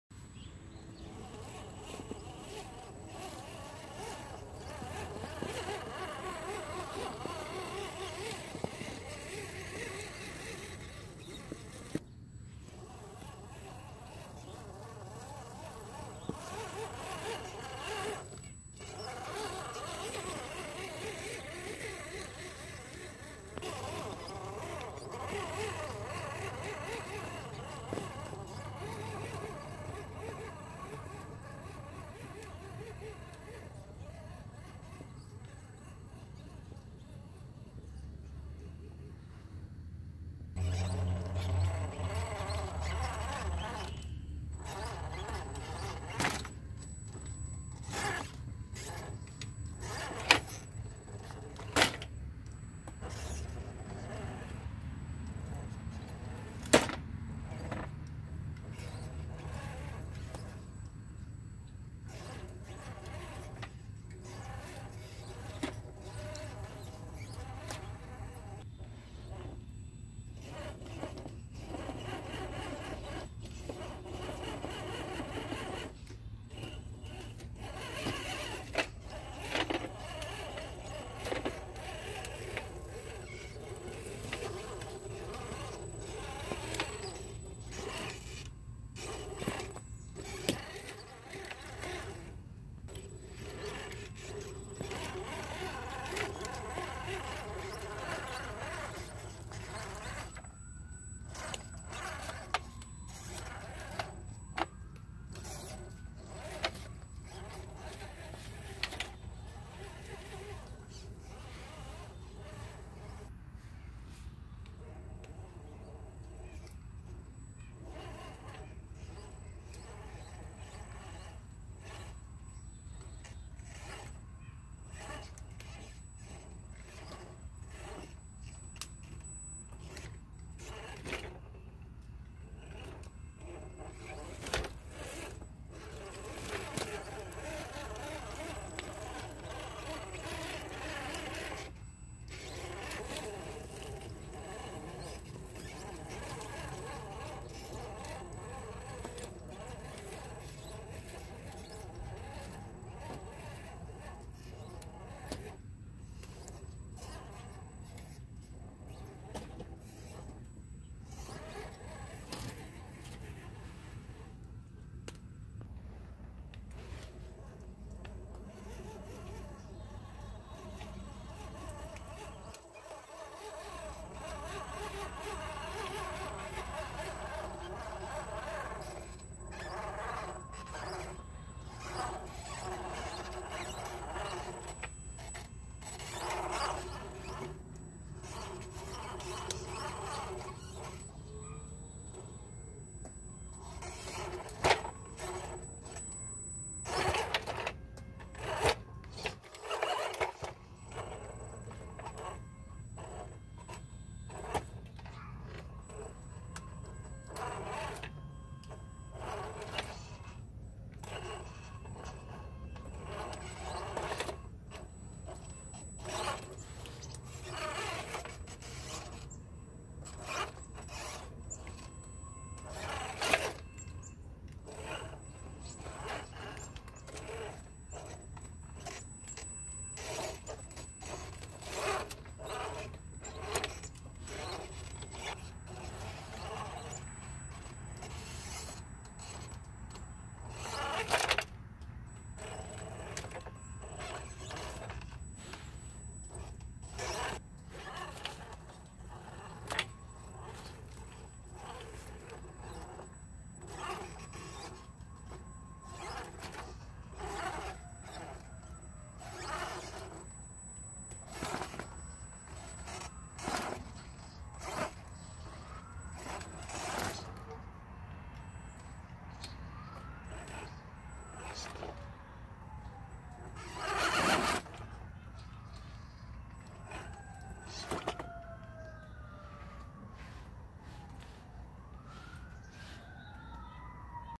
Rc4wd Toyota Hilux on my scale test course
I love how the suspension squeaks. One of my favorite rigs.